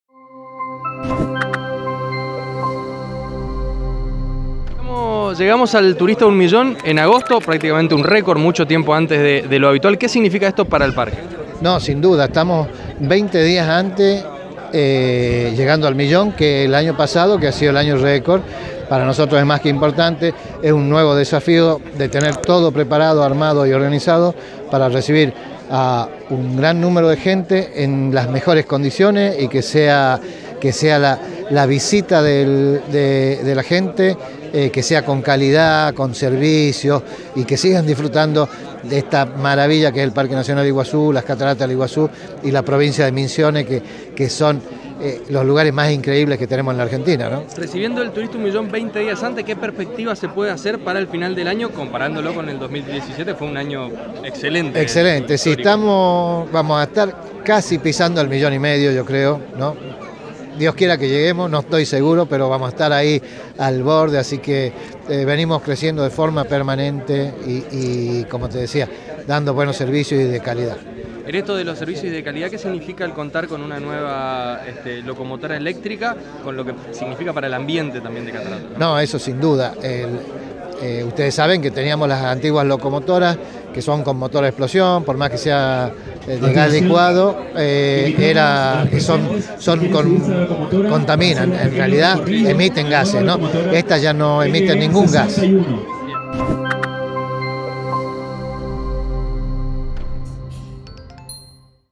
Sergio Valdecanto - Intendente Parque Cataratas del Iguazú
En este 2018, el turista un millón llegó 20 días antes que en 2017. Con este panorama, la perspectiva para este año en cataratas es más que auspiciosa